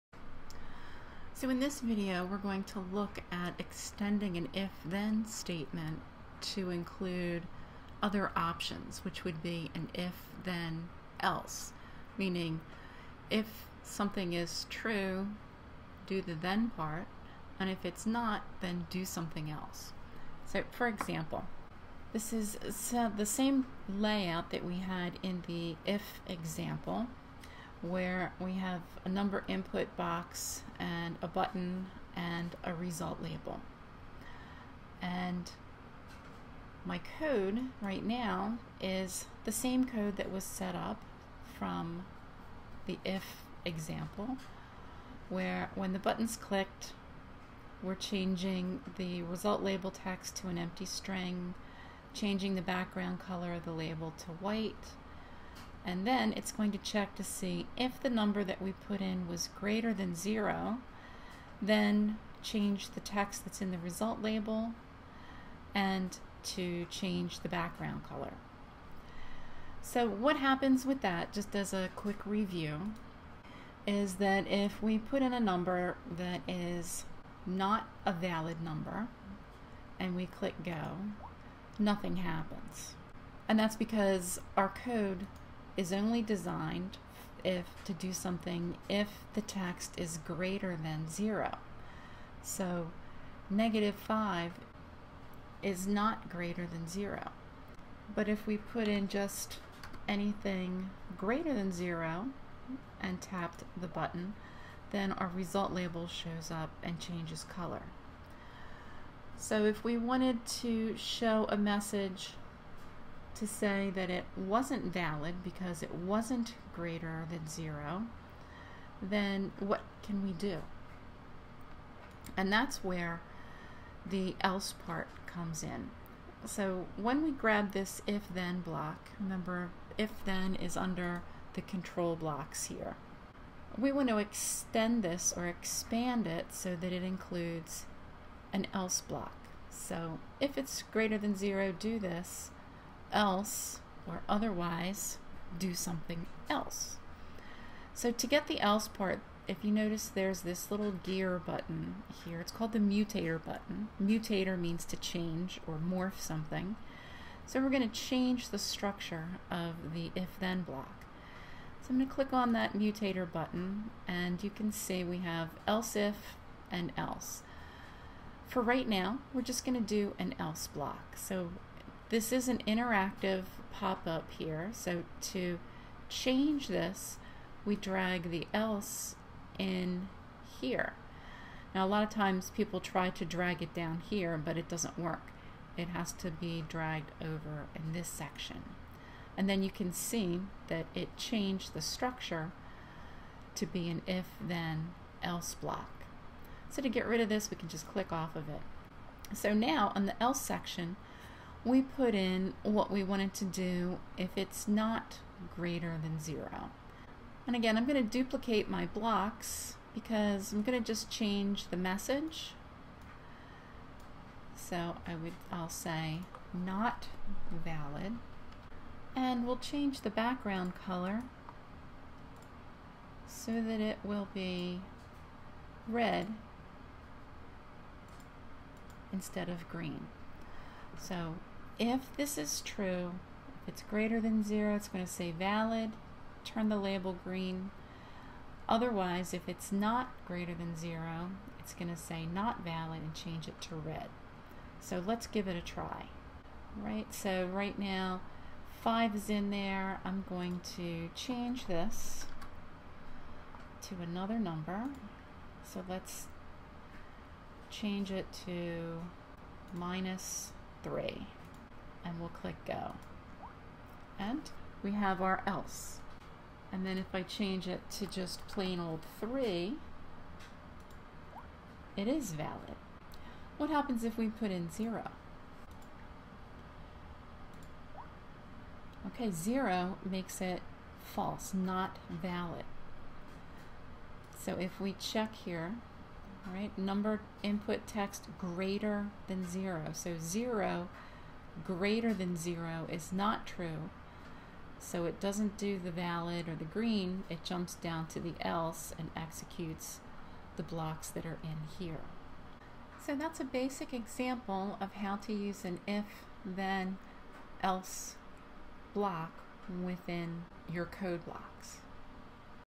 If-Then-Else Example
Demonstrates how to create and use an if-then-else structure in an app. and follow along.